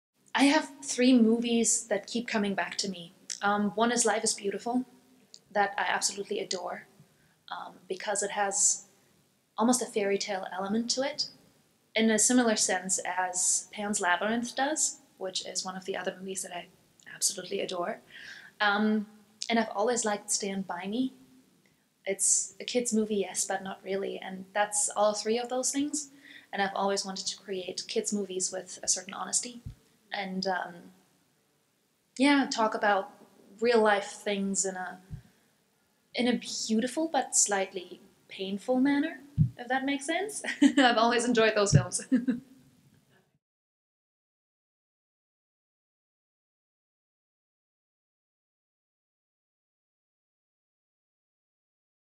Annika Pampel on Movies that Inspire Her
Annika Pampel was recently named one of the International Screenwriters’ Association’s Top 25 Screenwriters to Watch in 2019.  In this series of videos, we talk with the recent winner of Table Read My Screenplay about her early and current inspirations, personal goals and her tips for being a successful writer.